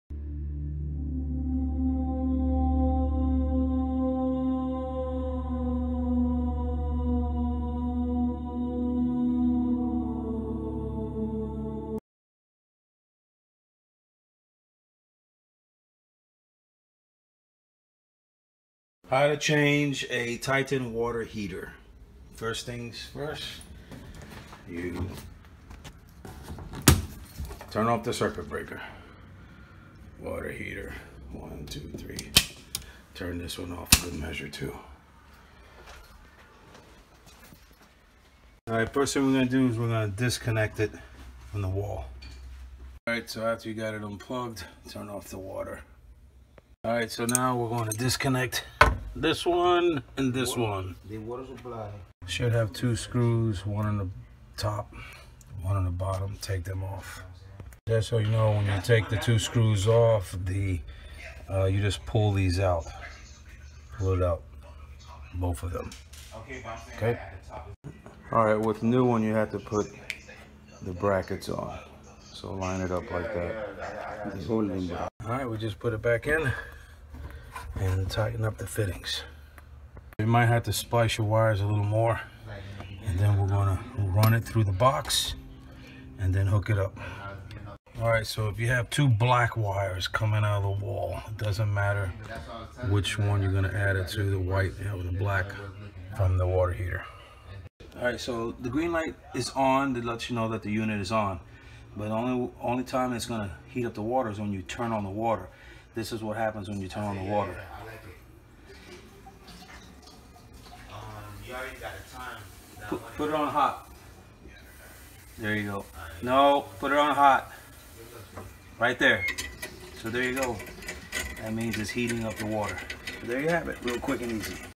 HOW TO INSTALL TITAN WATER HEATER IN YOUR HOME
If your old Titan water hear finally died, no worries.  with a couple common tools, you can change it yourself in 15 minutes.  The one I removed today lasted for 11 years.  Not bad.  Ok, so, enjoy.